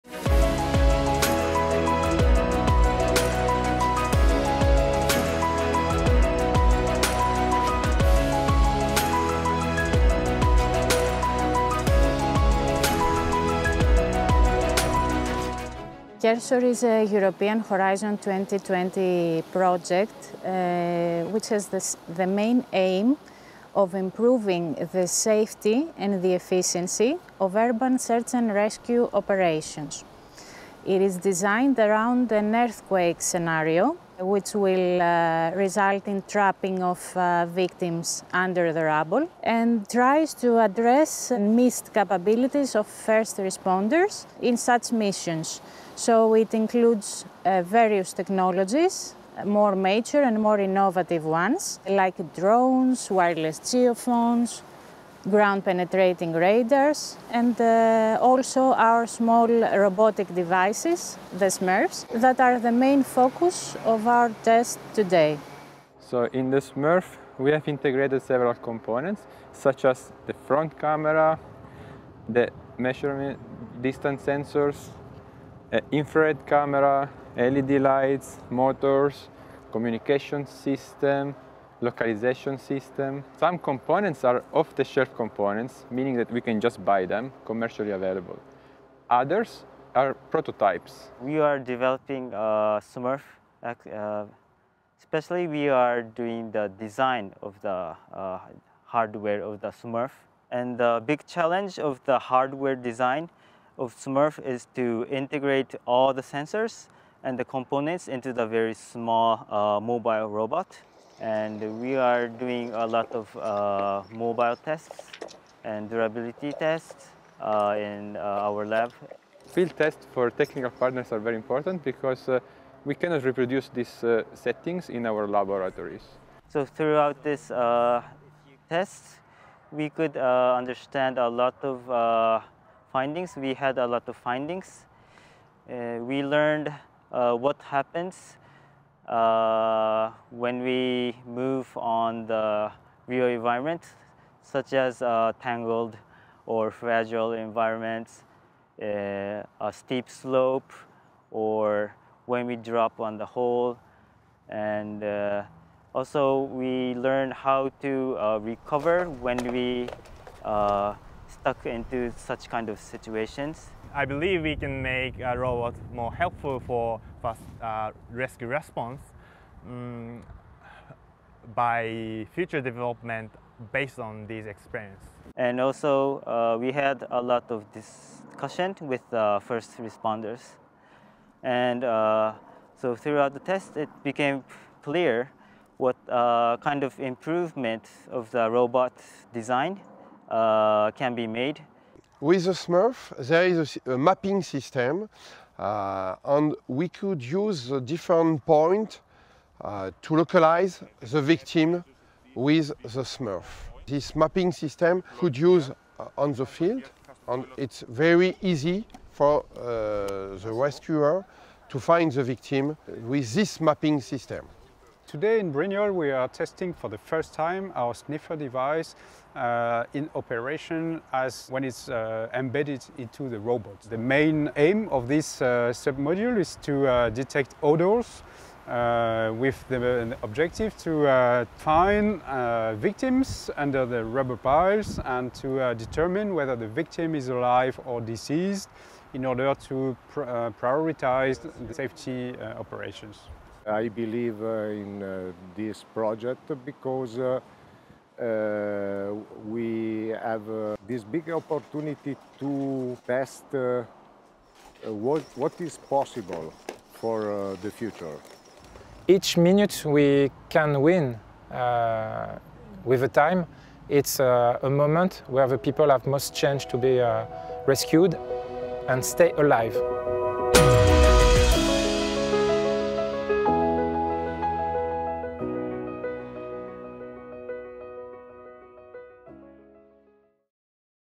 CURSOR Small Scale Field Test 1 2, SMURF, Brignoles, France, 28 - 30 September 2021
The first field assessment of the mini robot SMURF was held to test mobility capacities on different surfaces as well as the sensing capacities in realistic conditions, in particular using embedded cameras. The opportunity was taken to integrate Sniffer submodules into two SMURFs and to test them in such mobile conditions in the field. A Mesh network allowing communication features for the SMURF with the workstation was also deployed and evaluated.

Ten first responders from Italy and France could put their hand on the tools and drive the SMURFs on different surfaces and through obstacles. The SMURF sensors were also tested in the presence of volunteer’s “victims” and their output was observed on the workstation: video and infra-red cameras, and Sniffer raw data.

The CURSOR project is a new European research initiative which will develop a CURSOR Search and Rescue Kit using drones, miniaturised robotic equipment, and advanced sensors reducing time for detection and rescue of victims trapped under debris while increasing the personal safety of the SaR teams. Launched in September 2019 CURSOR is a project funded under the European Union’s Horizon 2020 research and innovation programme (grant agreement No 832790) and by the Japan Science and Technology Agency.